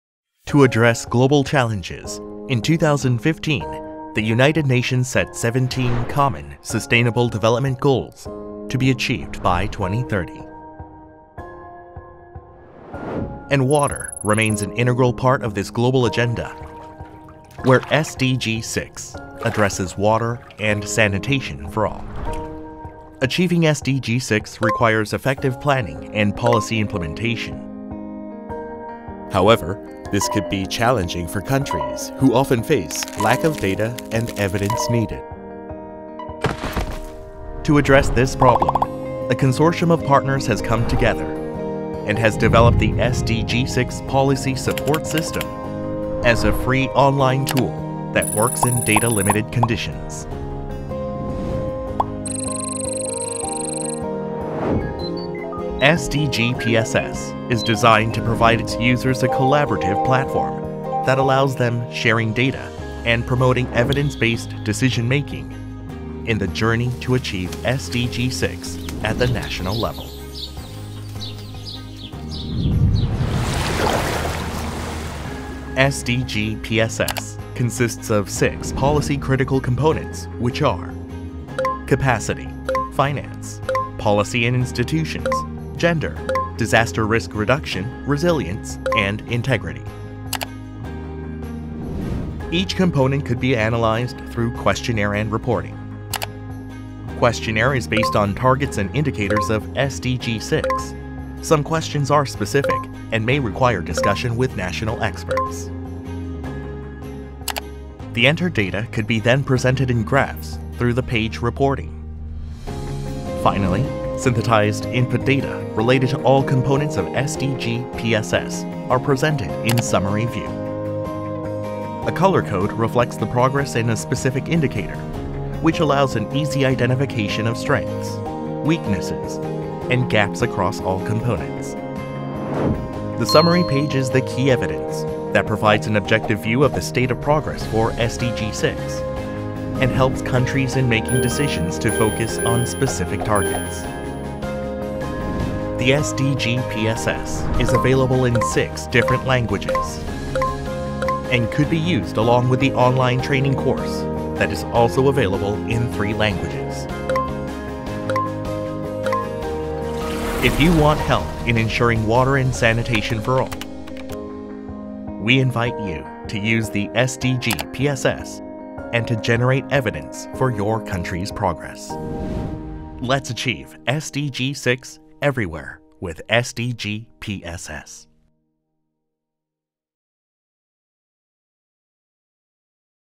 What is the SDG 6 Policy Support System (SDG-PSS)?
This tool was designed to generate evidence on the enabling environment of SDG 6 through a framework that shows strengths and weaknesses and missing data across all SDG 6 targets and indicators, and consists of the following components: Capacity Assessment; Finance; Policy and Institutional Assessment; Gender Mainstreaming; Disaster Risk Reduction (DRR)/Resilience Mainstreaming; and Integrity. These components are a result of a collaborative approach between project partners and were chosen as they allow a better understanding of the enabling environment for achieving SDG 6. SDG-PSS also monitors and evaluates another component of SDG 6, ‘Status’ – which gives an overview of trends for targets and indicators of the goal.

The tool and its online course for capacity building are currently available online in English, French, and Spanish languages.

The SDG 6 Policy Support System (SDG-PSS) is a collaborative project of United Nations Office for Sustainable Development (UNOSD) and the United Nations University Institute for Water, Environment and Health (UNU-INWEH), in partnership with the Ministry of Environment of Republic of Korea (MOE), Korea Environment Corporation (K-eco) and national partners from Tunisia, Costa Rica and Pakistan – the Regional Hubs countries.